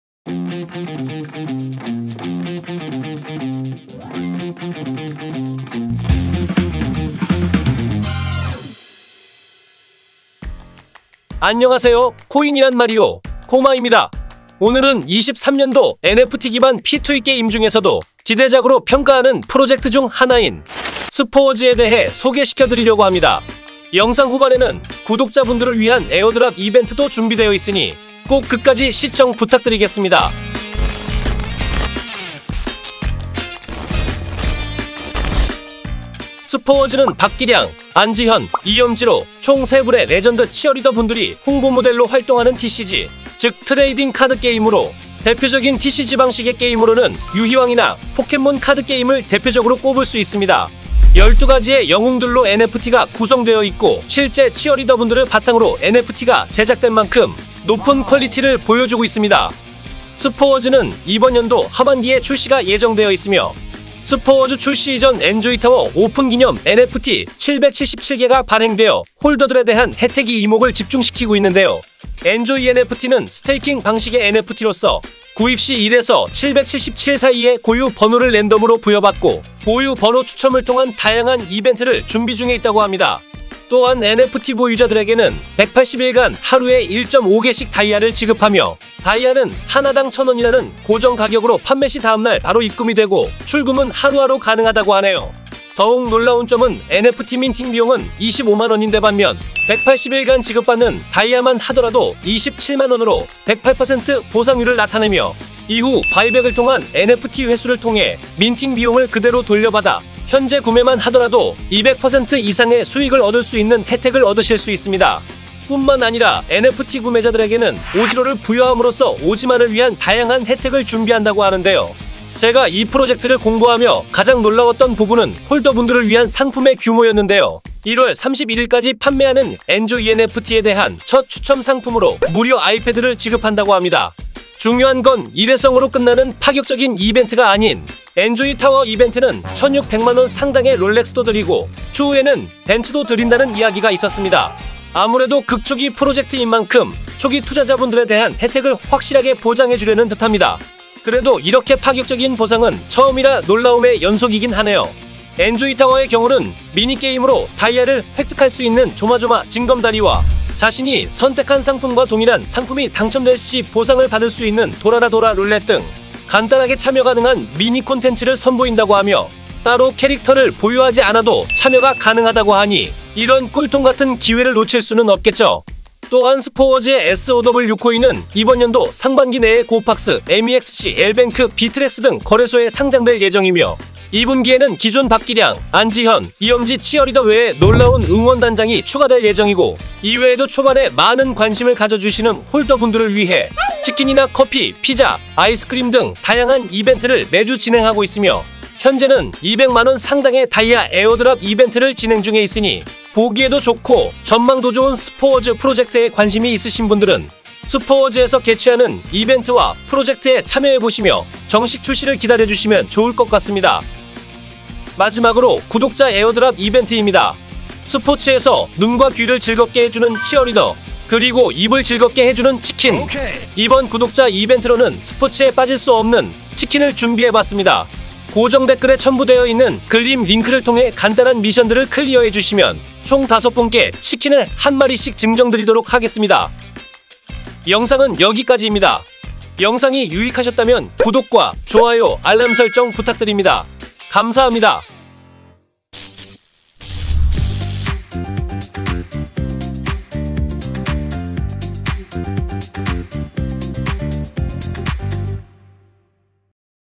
23년기대작 극초기 NFT프로젝트│스포워즈 레전드 치어리더들의 참여-에어드랍 이벤트까지!
무려 박기량, 안지현, 이엄지 치어리더님들이 직접 참여하신 프로젝트라니
규모가 정말 어마어마 한데요 이에 걸맞게 상품 라인업도 대단합니다..
영상 후반에는 구독자 이벤트도 있으니 꼭 참여해주세요~!

      한국어 지원, 수수료50%할인, 거래소 내의 다양한 혜택이 가득한 비트겟!
🤗 후오비글로벌 가입링크

⏱TIME TABLE
00:00 오픈톡 NFT 무료 에어드랍 이벤트
00:10 23년 기대작 스포워즈
00:36 스포워즈 소개
01:12 엔조이NFT 수익 보상 시스템
02:35 엔조이타워 플레이방법
02:56 스포워즈 업데이트 계획
03:34 구독자 에어드랍 이벤트
04:05 아웃트로

[📺추천 영상]
레타워즈 주니어NFT│주니어 하나하나 파헤쳐보자~!-2차 NFT에어드랍 이벤트

오늘은 23년도 NFT기반 P2E게임 중에서도 기대작으로 평가하는 프로젝트 중 하나인 스포워즈에 대해 소개시켜드리려고 합니다.
영상 후반에는 구독자분들을 위한 에어드랍 이벤트도 준비되어 있으니 꼭 끝까지 시청 부탁드리겠습니다.

스포워즈는 박기량, 안지현, 이엄지로 총 3분의 레전드 치어리더 분들이 홍보모델로 활동하는 TCG, 즉 Trading Card Game으로 대표적인 TCG 방식의 게임으로는 유희왕이나 포켓몬 카드 게임을 대표적으로 꼽을 수 있습니다.
12가지의 영웅들로 NFT가 구성되어 있고, 실제 치어리더분들을 바탕으로 NFT가 제작된 만큼 높은 퀄리티를 보여주고 있습니다.

스포워즈는 이번년도 하반기에 출시가 예정되어 있으며, 스포워즈 출시이전 엔조이타워 오픈기념 NFT 777개가 발행되어 홀더들에 대한 혜택이 이목을 집중시키고 있는데요

엔조이NFT는 스테이킹 방식의 NFT로써, 구입시 1~777 사이의 고유 번호를 랜덤으로 부여받고, 고유 번호 추첨을 통한 다양한 이벤트를 준비중에 있다고 합니다. 또한 NFT보유자들에게는 180일간 하루에 1.5개씩 다이아를 지급하며, 다이아는 하나당 1000원이라는 고정 가격으로 판매시 다음날 바로 입금이 되고, 출금은 하루하루 가능하다고 하네요

더욱 놀라운점은 NFT민팅비용은 25만원인데 반면, 180일간 지급받는 다이아만 하더라도 27만원으로 108% 보상율을 나타내며, 이후 바이백을 통한 NFT 회수를 통해 민팅비용을 그대로 돌려받아 현재 구매만 하더라도 200% 이상의 수익을 얻을 수 있는 혜택을 얻으실 수 있습니다.
뿐만 아니라 NFT 구매자들에게는 OG롤을 부여함으로써 OG만을 위한 다양한 혜택을 준비한다고 하는데요
제가 이 프로젝트를 공부하며 가장 놀라웠던 부분은 홀더분들을 위한 상품의 규모였는데요 1월 31일까지 판매하는 ENJOY NFT에 대한 첫 추첨 상품으로 무려 아이패드를 지급한다고 합니다.
중요한건 1회성으로 끝나는 파격적인 이벤트가 아닌 엔조이 타워 이벤트는 1,600만원 상당의 롤렉스도 드리고 추후에는 벤츠도 드린다는 이야기가 있었습니다.
아무래도 극초기 프로젝트인만큼, 초기 투자자분들에 대한 혜택을 확실하게 보장해주려는 듯합니다. 그래도 이렇게 파격적인 보상은 처음이라 놀라움의 연속이긴 하네요

엔조이타워의 경우는 미니게임으로 다이아를 획득할 수 있는 조마조마 징검다리와 자신이 선택한 상품과 동일한 상품이 당첨될 시 보상을 받을 수 있는 돌아라 돌아 룰렛 등 간단하게 참여가능한 미니콘텐츠를 선보인다고 하며, 따로 캐릭터를 보유하지 않아도 참여가 가능하다고 하니, 이런 꿀통같은 기회를 놓칠수는 없겠죠?

또한 스포워즈의 SOW코인은 이번년도 상반기 내에 GOPAX, MEXC, LBANK, BITTREX,등 거래소에 상장될 예정이며, 2분기에는 기존 박기량, 안지현, 이엄지 치어리더 외에 놀라운 응원단장이 추가될 예정이고, 이외에도 초반에 많은 관심을 가져주시는 홀더분들을 위해 치킨이나 커피, 피자, 아이스크림 등 다양한 이벤트를 매주 진행하고 있으며, 현재는 200만원 상당의 다이아 에어드랍 이벤트를 진행중에 있으니, 보기에도 좋고 전망도 좋은 스포워즈 프로젝트에 관심이 있으신분들은 스포워즈에서 개최하는 이벤트와 프로젝트에 참여해보시며 정식 출시를 기다려주시면 좋을 것 같습니다.

마지막으로 구독자 에어드랍 이벤트입니다.
스포츠에서 눈과 귀를 즐겁게 해주는 치어리더, 그리고 입을 즐겁게 해주는 치킨!
이번 구독자 이벤트로는 스포츠에 빠질 수 없는 치킨을 준비해봤습니다.
고정댓글에 첨부되어 있는 Gleam 링크를 통해 간단한 미션들을 클리어해주시면 총 다섯분께 치킨을 한 마리씩 증정드리도록 하겠습니다!